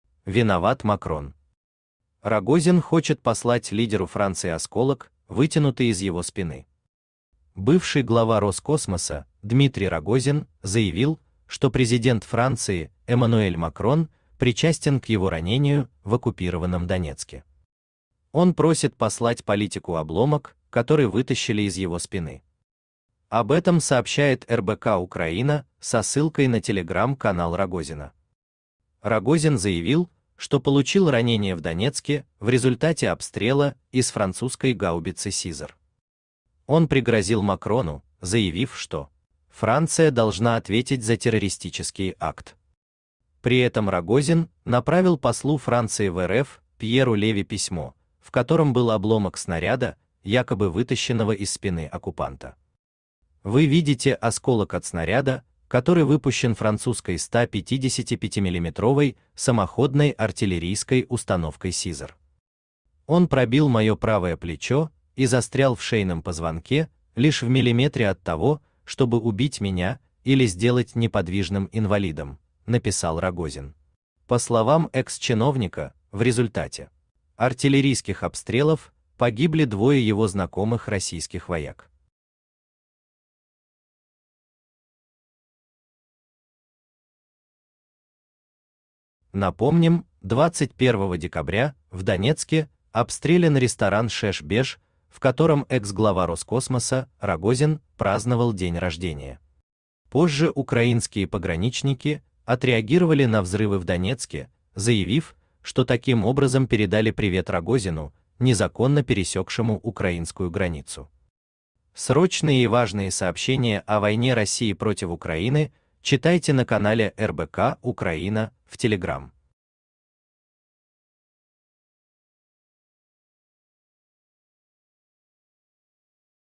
"Виноват Макрон". Рогозин хочет послать лидеру Франции осколок, вытянутый из его спины.
Рогозин Роскосмос Франция ЭммануэльМакрон ВойнавУкраине Бывший глава "Роскосмоса" Дмитрий Рогозин заявил, что президент Франции Эммануэль Макрон причастен к его ранению в оккупированном Донецке. Он просит послать политику обломок, который вытащили из его спины. Об этом сообщает РБК-Украина со ссылкой на Telegram-канал Рогозина. Рогозин заявил, что получил ранения в Донецке в результате обстрела из французской гаубицы Caesar. Он пригрозил Макрону, заявив, что Франция должна ответить за "террористический акт". При этом Рогозин направил послу Франции в РФ